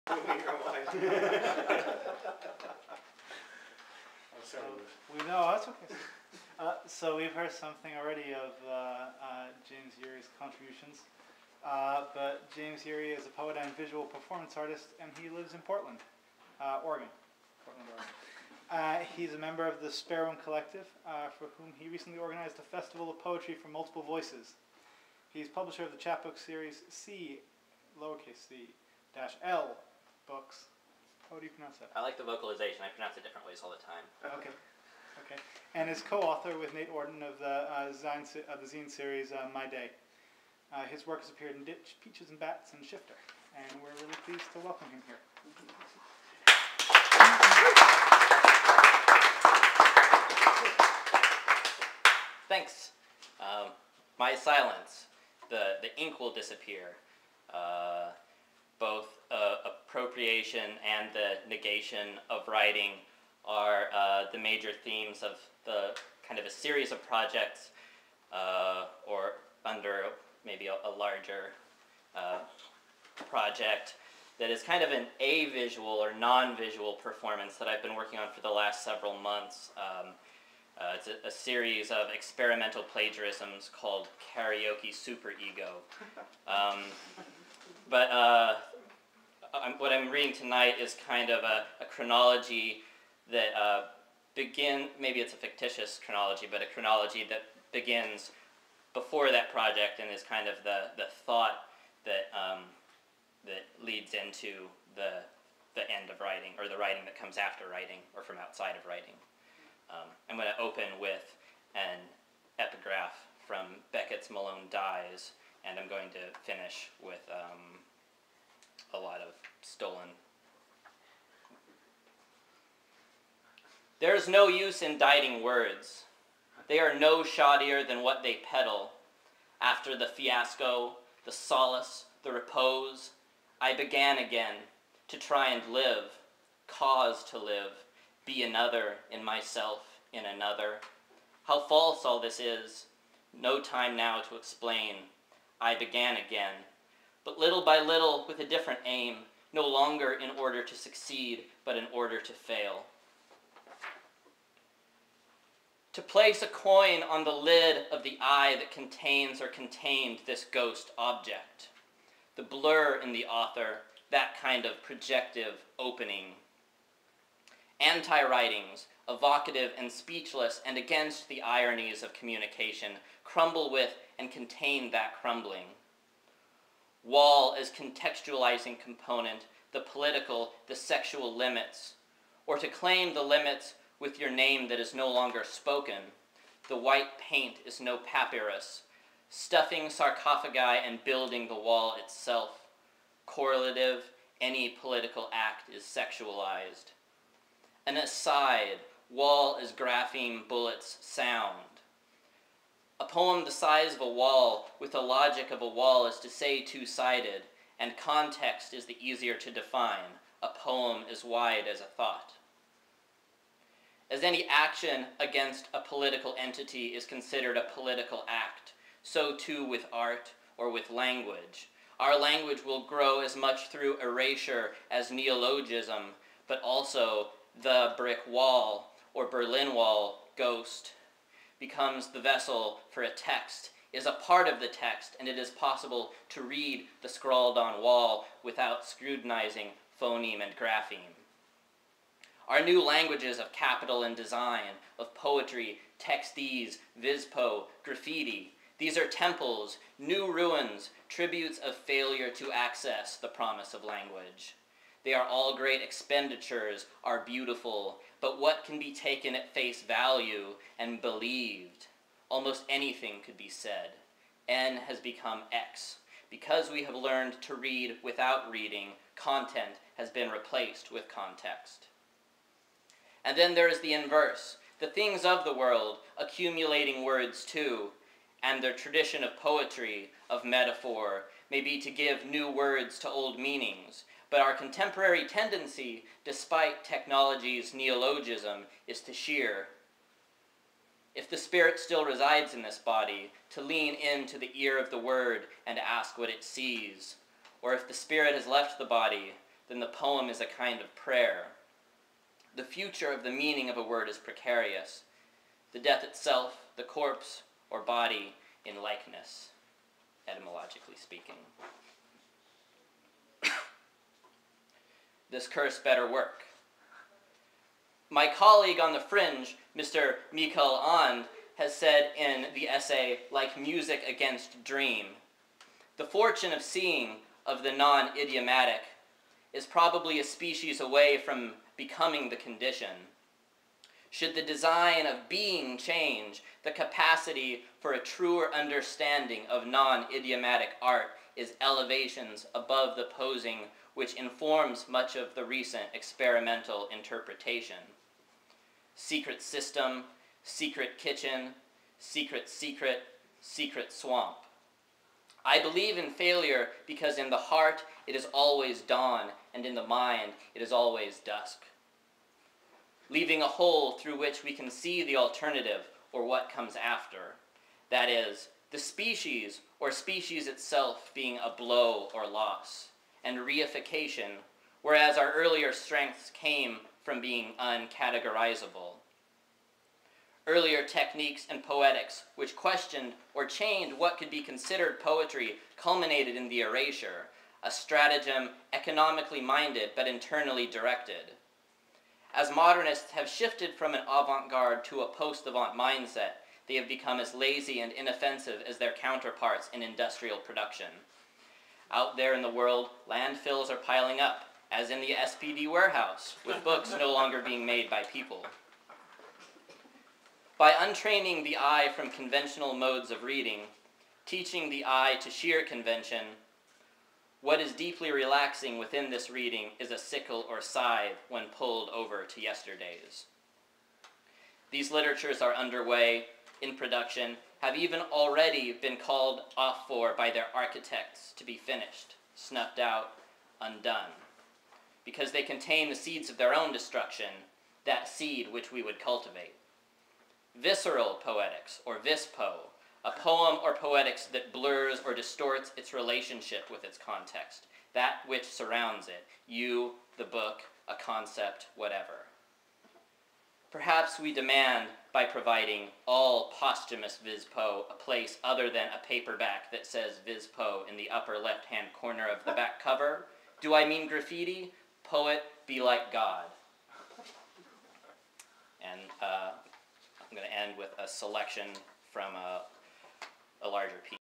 james yeary 1 of 2
James Yeary reads at Avol's Books, Madison. James is a poet and visual/performance artist living in Portland, Oregon. He is a member of the Spare Room collective for whom he recently organized a festival of poetry for multiple voices. He is publisher of the chapbook series c_L books and is co-author (with Nate Orton) of the zine series my day. His work has appeared in ditch, Peaches & Bats, and SHIFTER.